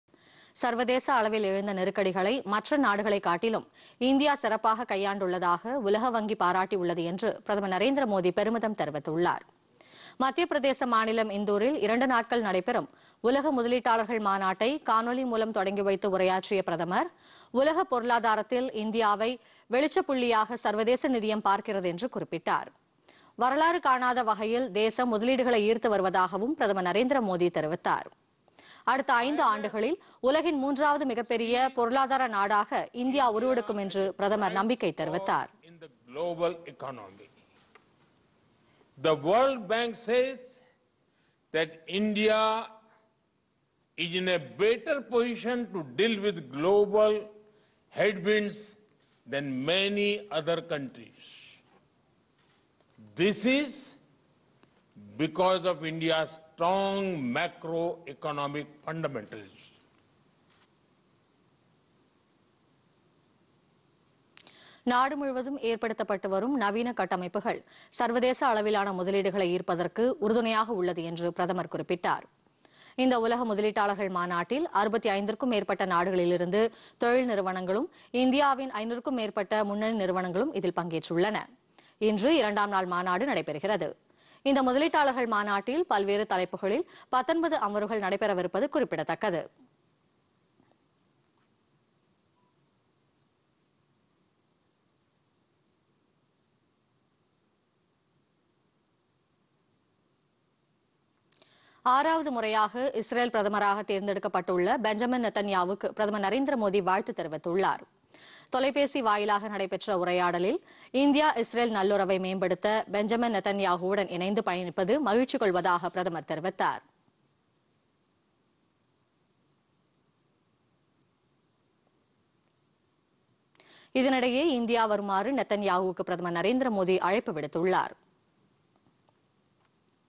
சர்வதேச முதலீட்டாளர்கள் மாநாடு
சர்வதேச அளவில் எழுந்த நெருக்கடிகளை சமாளித்த  இந்தியாவுக்கு உலக வங்கி  பாராட்டு-  சர்வதேச முதலீட்டாளர்கள் மாநாட்டில் பிரதமர் நரேந்திர மோதி ‍பெருமிதம்.